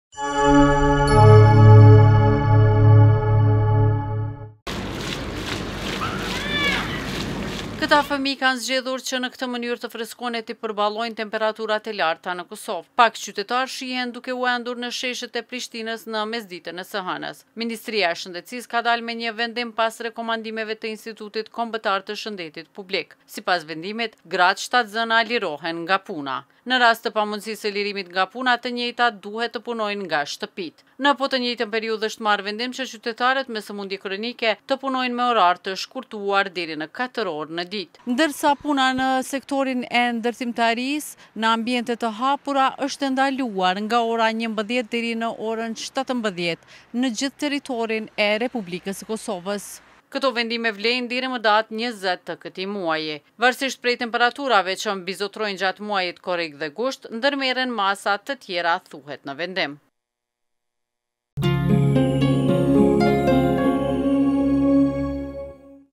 Tv Klan - Kosova përfshihet nga i nxehti afrikan, qeveria merr masa lehtësuese për punonjësit
Këta fëmijë kanë zgjedhur që në këtë mënyrë të freskohen e t’i përballojnë temperaturat e larta në Kosovë.

Pak qytetarë shihen duke u endur në sheshet e Prishtinës në mesditën e së hënës.

Ministria e Shëndetësisë ka dalë me një vendim pas rekomandimeve të Instituti Kombëtar të Shëndetit Publik.

Sipas vendimit, gratë shtatzëna lirohen nga puna. Në rast të pamundësisë së lirimit nga puna të njëjtat duhet të punojnë nga shtëpitë.

Në po të njëjtën periudhë, është marrë vendim që qytetarët me sëmundje kronike të punojnë me orar të shkurtuar deri në 4 orë në ditë.

Gjithashtu ndalohet puna në sektorin e ndërtimtarisë në ambiente të hapura gjatë orarit 11:00 – 17:00 në të gjithë territorin e Republikës së Kosovës.

Këto vendime vlejnë deri më datën 20 të këtij muaji. Varësisht prej temperaturave që mbizotërojnë gjatë muajit Korrik dhe Gusht 2024, ndërmerren masa të tjera, thuhet në vendim.